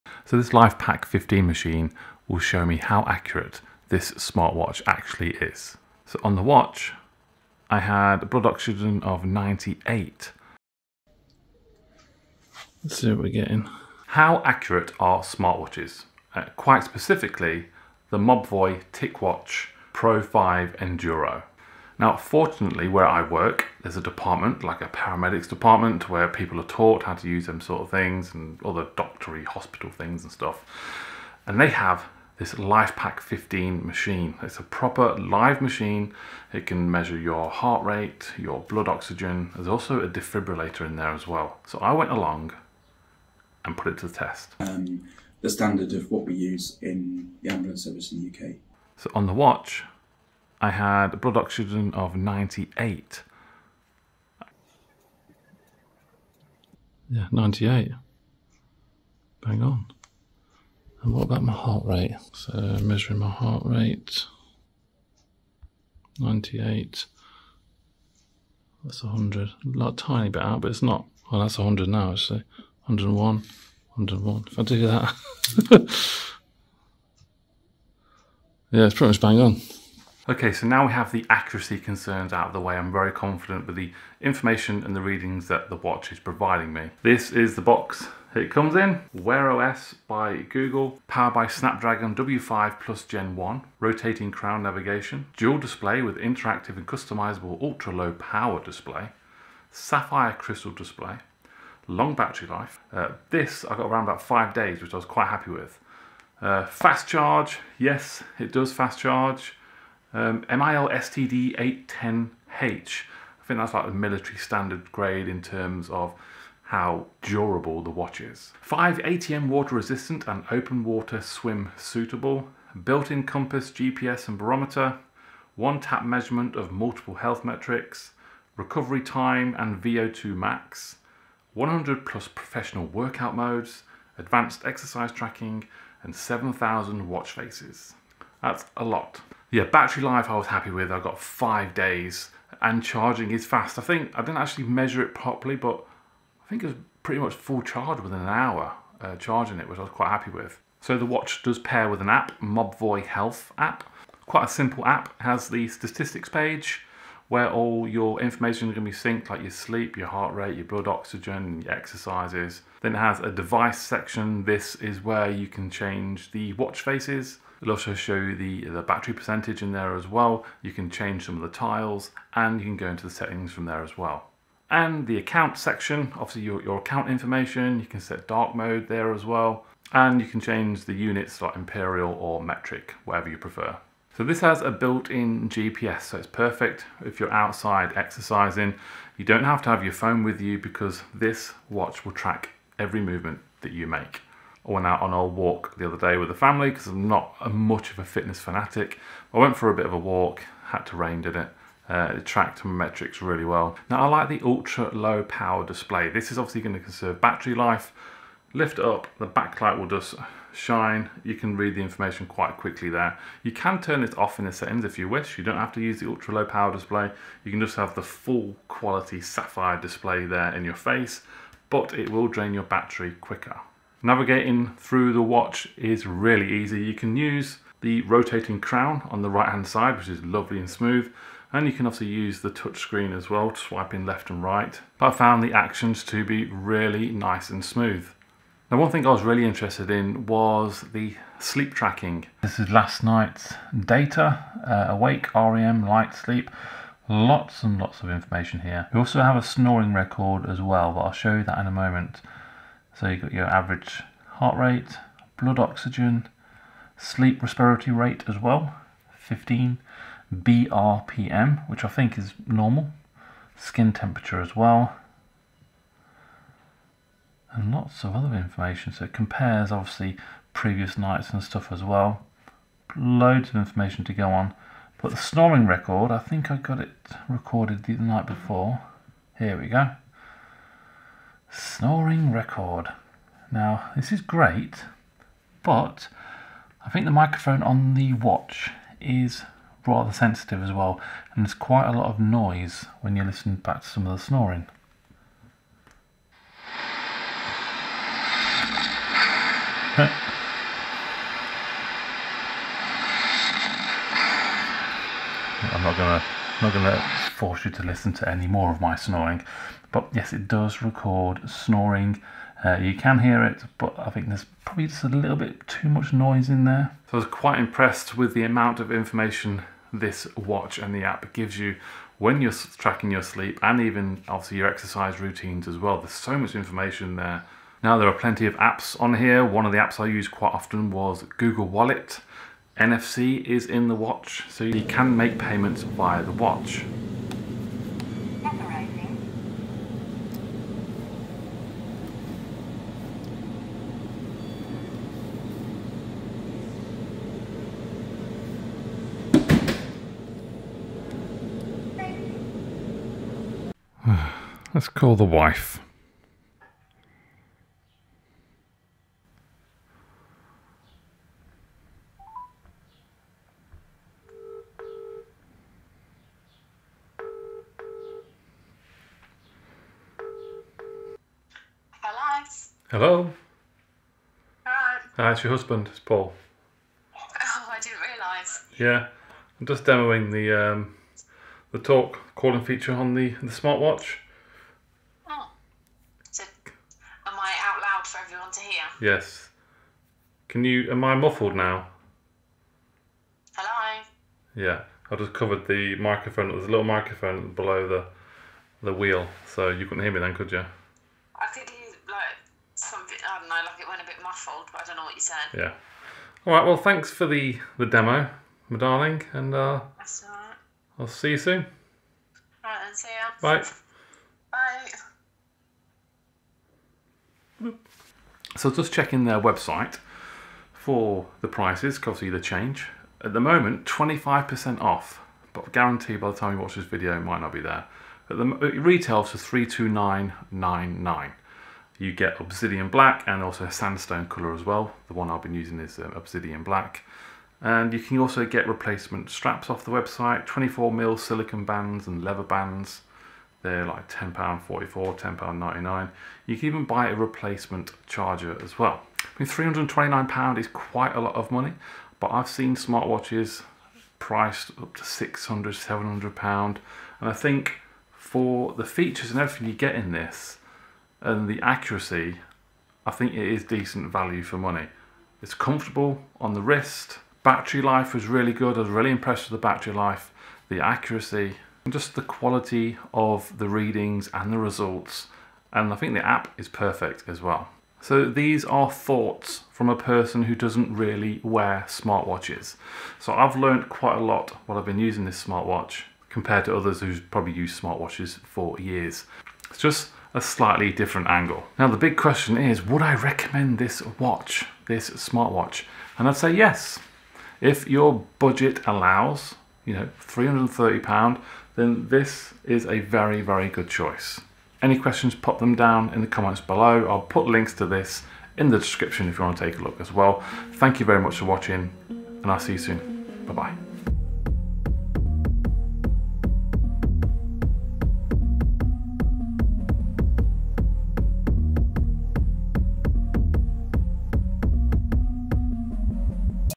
TicWatch Pro 5 Enduro vs Lifepack 15 Machine
How Accurate Is Your Smartwatch ? TicWatch Pro 5 Enduro - I test the accuracy of heart rate and blood oxygen using a Lifepack 15 Machine, professional equipment used in the UK ambulance service. Smartwatches are great, but I am always curious as to the accuracy of the readings.